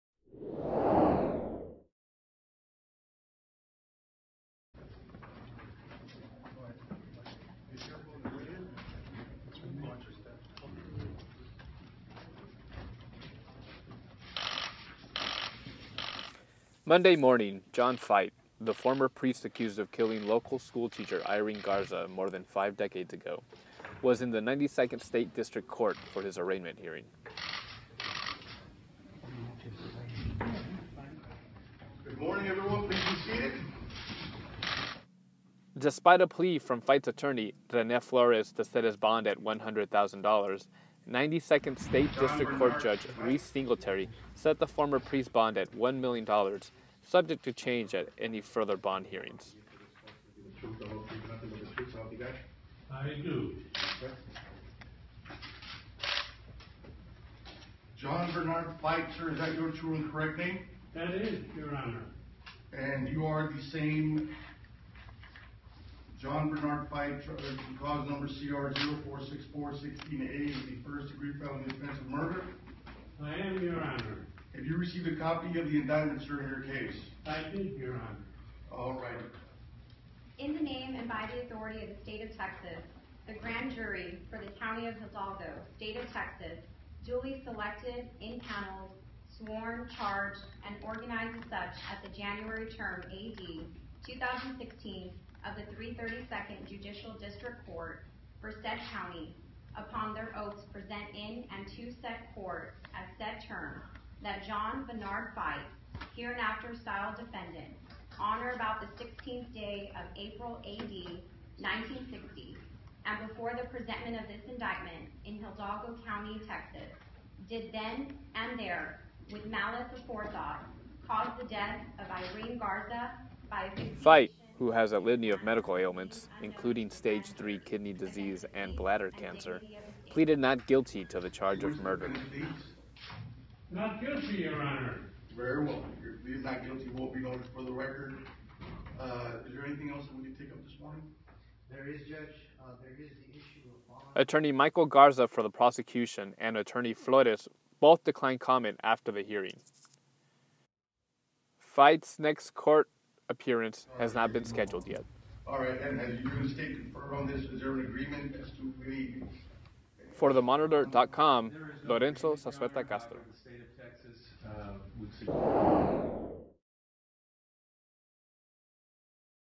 John Feit Arraignment Hearing March 14, 2016
Former priest John Feit, who is accused of killing school teacher Irene Garza more than five decades ago, was formally arraigned in the 92nd State District Court March 14, 2016. Feit's next scheduled hearing has not been set.